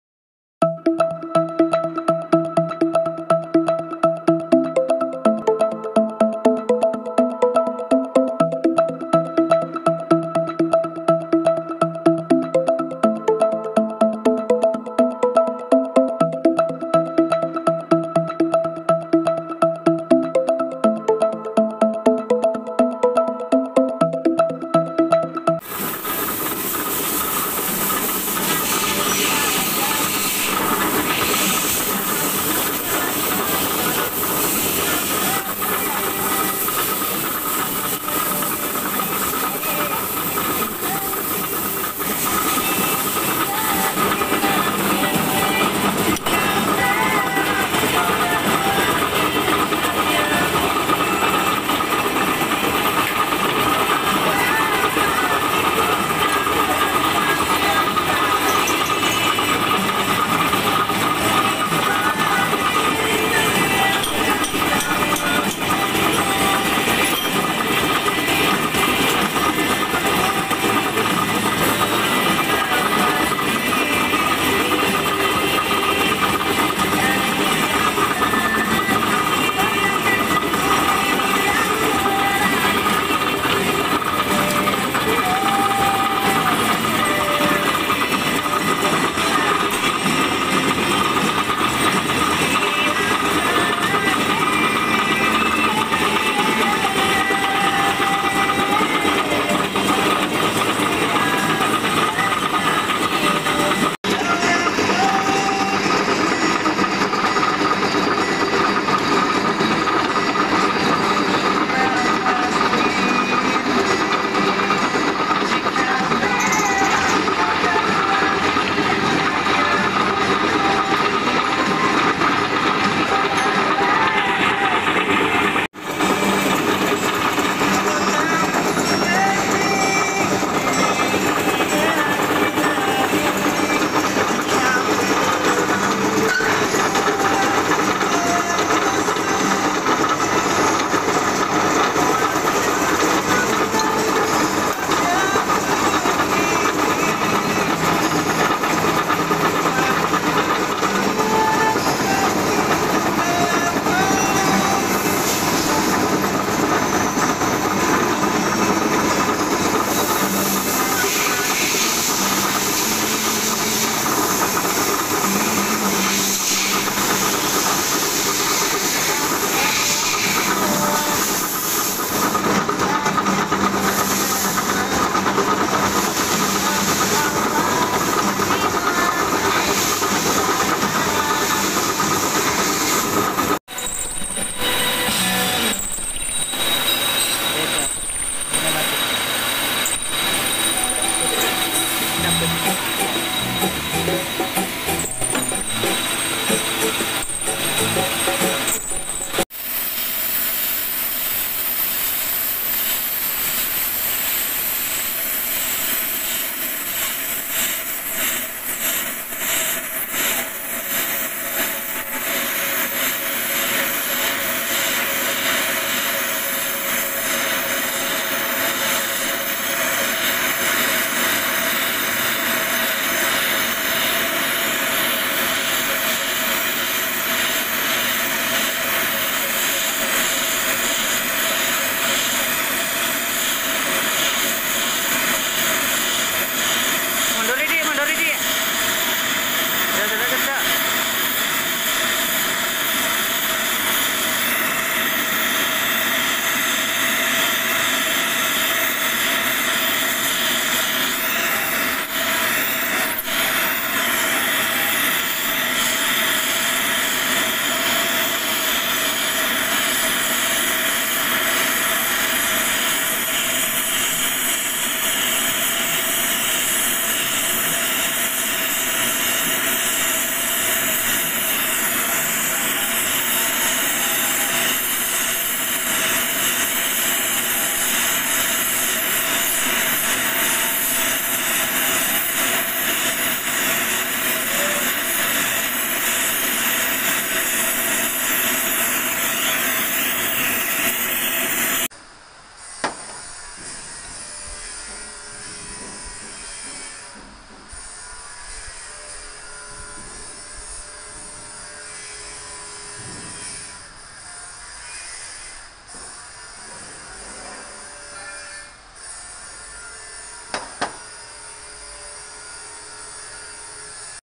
Proses Buat Lubang Segi 6 /How to make Hexagon hole
Proses buat lubang segi enam,buat lubang kunci L